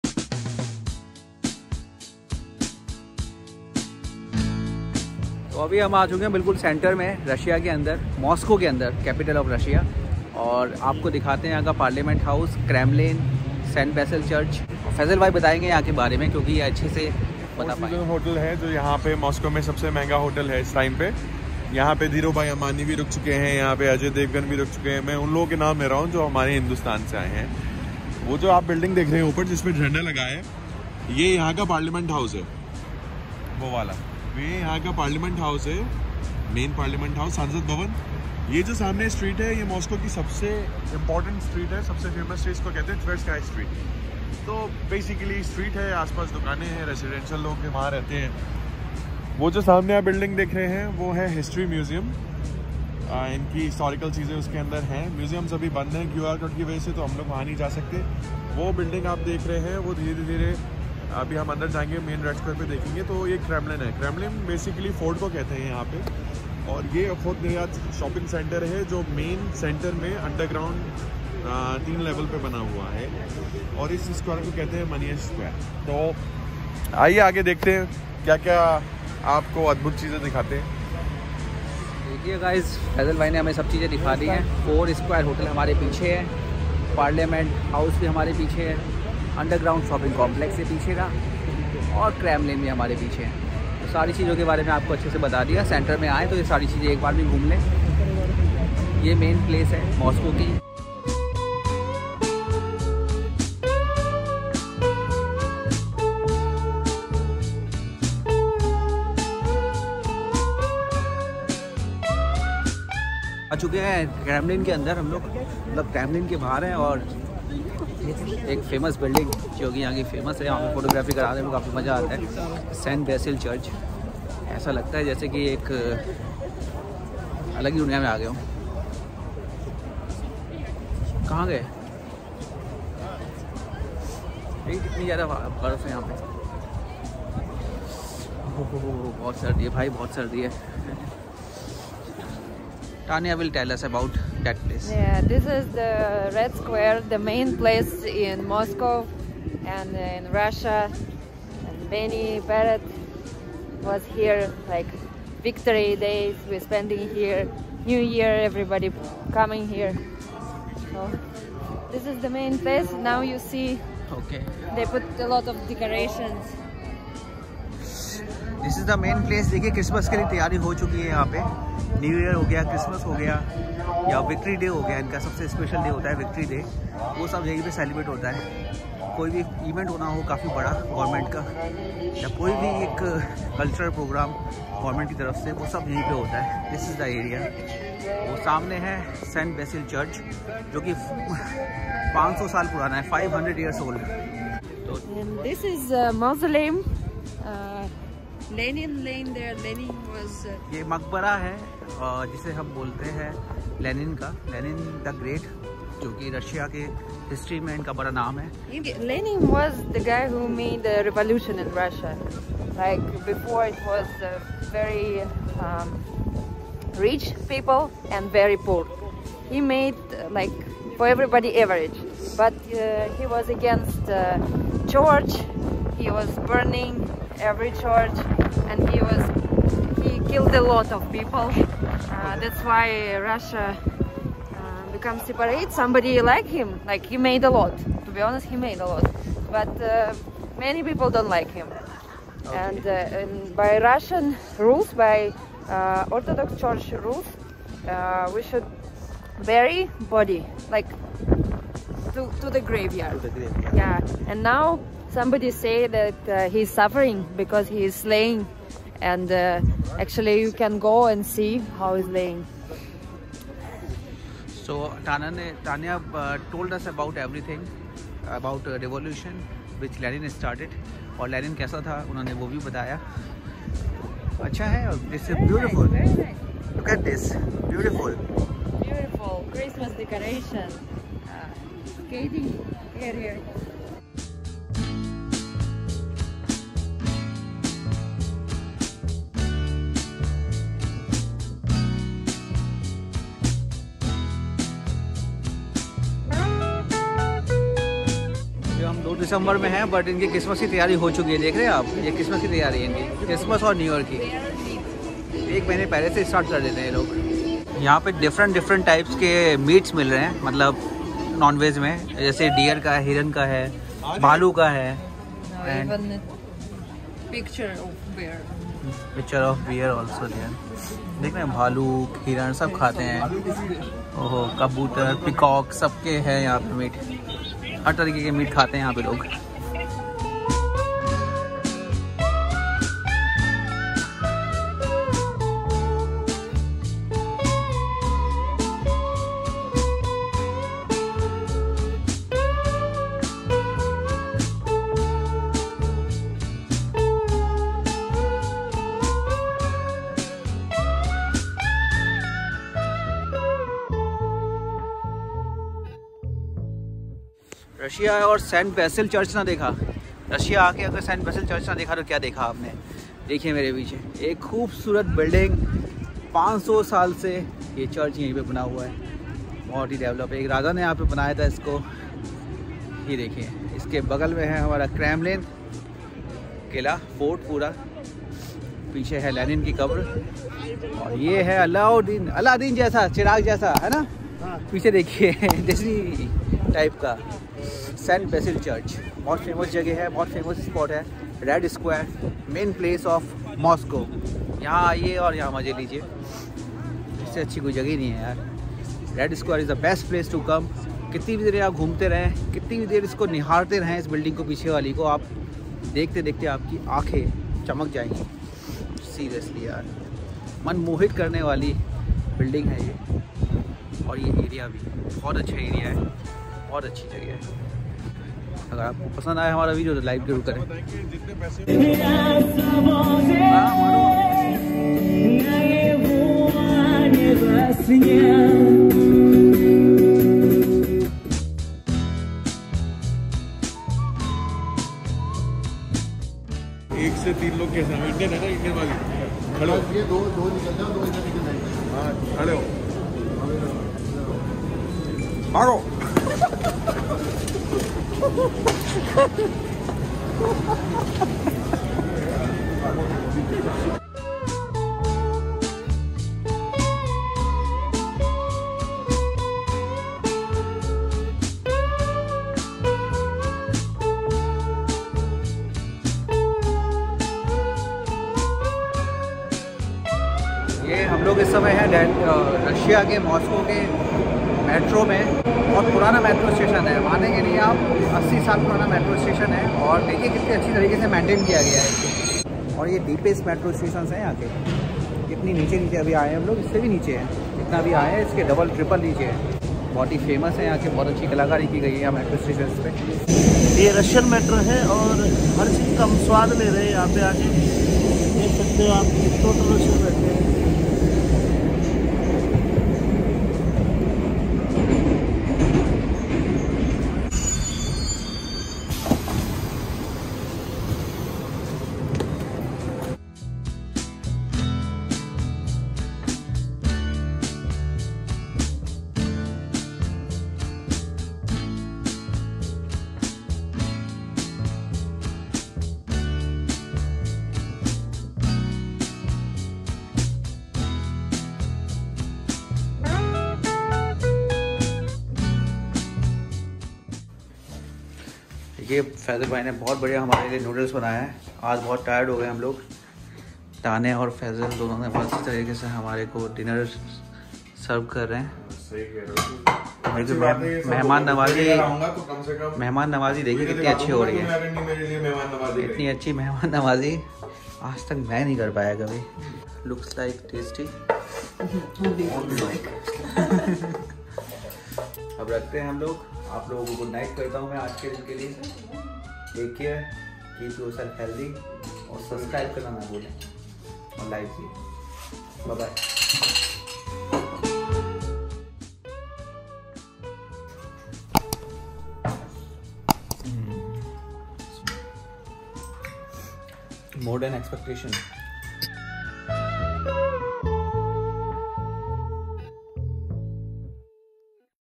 Vlog-09 | Moscow ke central area aurr metro mein ghumte hue 🇷🇺
We roam around the central area of Moscow that covers palace, president house and St. Basil Church that is 500yrs old. We saw the malls and streets that has a number of shopping areas.

We also showed various metros of Russia that are world's deepest stations. Also, they are very old metro stations.